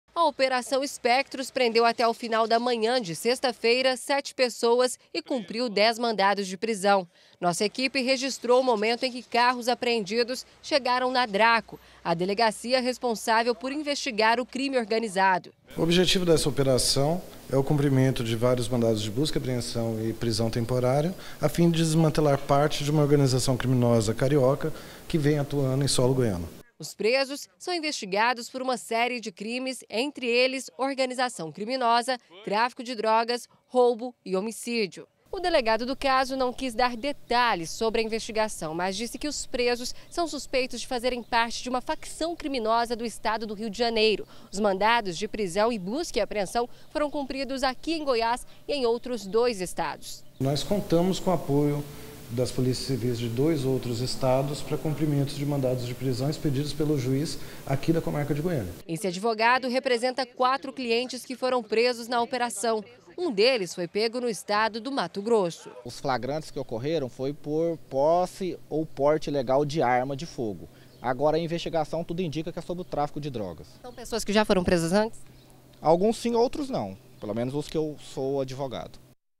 BG - Polícia civil combate crime organizado - 03-08-2018
A polícia civil realizou hoje uma operação para impedir a ação de uma quadrilha carioca aqui em Goiás. Foram apreendidas armas e dinheiro.